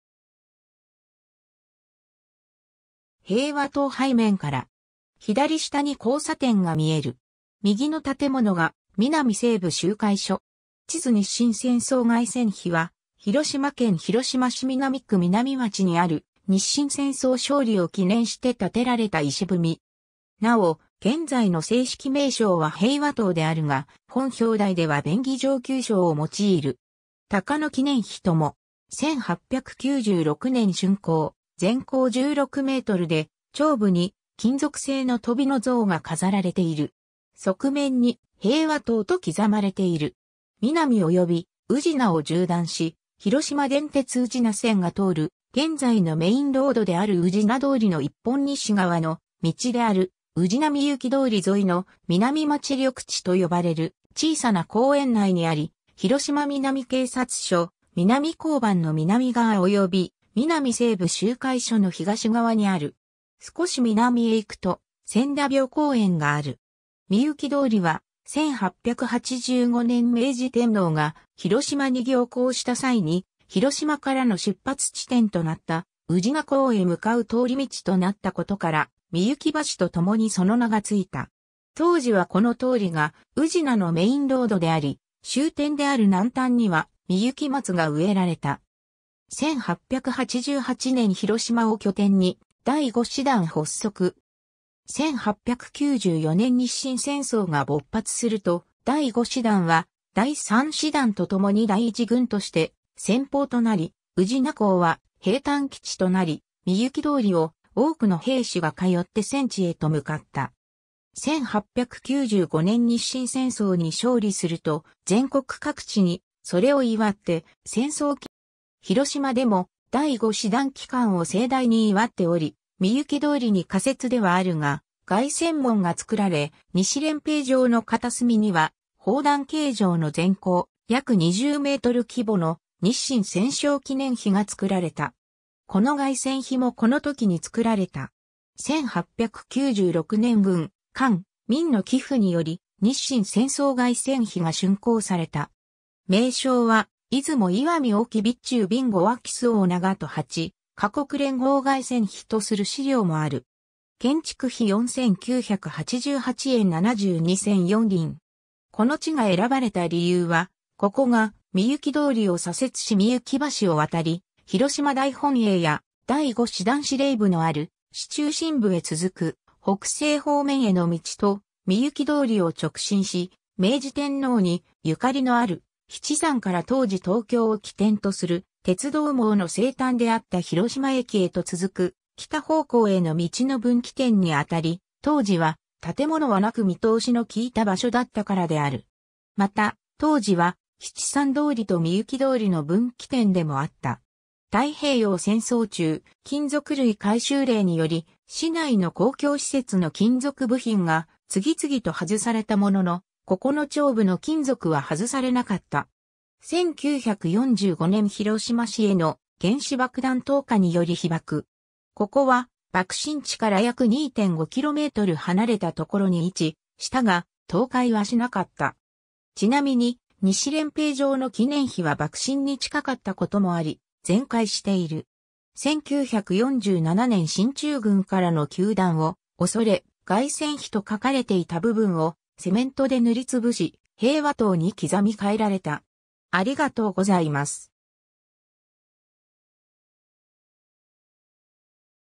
日清戦争凱旋碑
皆実町_(広島市)
平和塔(日清戦争凱旋碑) 背面から。
左下に交差点が見える（歴史部分参照）。
右の建物が皆実西部集会所。
 地図 日清戦争凱旋碑（にっしんせんそうがいせんひ）は、広島県広島市南区皆実町にある、日清戦争勝利を記念して建てられた碑。
なお、現在の正式名称は平和塔であるが、本表題では便宜上旧称を用いる。
鷹の記念碑とも。
 1896年（明治29年）竣工、全高16メートルで、頂部に金属製のトビ（金鵄）の像が飾られている。
側面に「平和塔」と刻まれている。
 皆実および宇品を縦断し広島電鉄宇品線が通る現在のメインロードである宇品通り（国道487号）の一本西側の道である「宇品御幸通り」（市道南4区557号線）沿いの、「皆実町緑地」と呼ばれる小さな公園内にあり、広島南警察署皆実交番の南側および皆実西部集会所の東側にある。
 少し南へ行くと千田廟公園がある。
 御幸通りは、1885年（明治18年）明治天皇が広島に行幸した際に、広島からの出発地点となった宇品港（現広島港）へ向かう通り道となったことから、御幸橋とともにその名がついた。
当時はこの通りが宇品のメインロードであり、終点である南端には御幸松が植えられた。
 1888年（明治21年）広島を拠点に第5師団発足。
1894年（明治27年）日清戦争が勃発すると、第5師団は第3師団とともに第1軍として先鋒となり、宇品港は兵站基地となり、御幸通りを多くの兵士が通って戦地へと向かった。
 1895年（明治28年）日清戦争に勝利すると、全国各地にそれを祝って戦争祈念施設である記念碑・凱旋門・凱旋碑・忠魂碑が造られた。
広島でも第5師団帰還を盛大に祝っており、御幸通りに仮設ではあるが凱旋門が作られ、西練兵場（現中区基町の広島城南側、旧広島市民球場から広島県庁舎一帯）の片隅には砲弾形状の全高約20メートル規模の「日清戦勝記念碑」が造られた。
この凱旋碑もこの時に造られた。
 1896年（明治29年）軍・官・民の寄付により「日清戦争凱旋碑」が竣工された。
名称は「出雲石見隠岐備中備後安芸周防長門八カ国連合凱旋碑」とする資料もある。
建築費4,988円72銭4厘。
この地が選ばれた理由は、ここが御幸通りを左折し御幸橋を渡り広島大本営や第5師団司令部のある市中心部へ続く北西方面への道と、御幸通りを直進し明治天皇にゆかりのある比治山から当時東京を起点とする鉄道網の西端であった広島駅へと続く北方向への道の分岐点にあたり、当時は建物はなく見通しの利いた場所だったからである。
また、当時は比治山通りと御幸通りの分岐点でもあった。
 太平洋戦争中、金属類回収令により市内の公共施設の金属部品が次々と外されたものの、ここの頂部の金属は外されなかった。
 1945年（昭和20年）広島市への原子爆弾投下により被爆。
ここは爆心地から約2.5キロメートル離れたところに位置したが、倒壊はしなかった。
ちなみに、西練兵場の記念碑は爆心に近かったこともあり全壊している。
 1947年（昭和22年）進駐軍からの糾弾を恐れ、「凱旋碑」と書かれていた部分をセメントで塗りつぶし「平和塔」に刻み変えられた。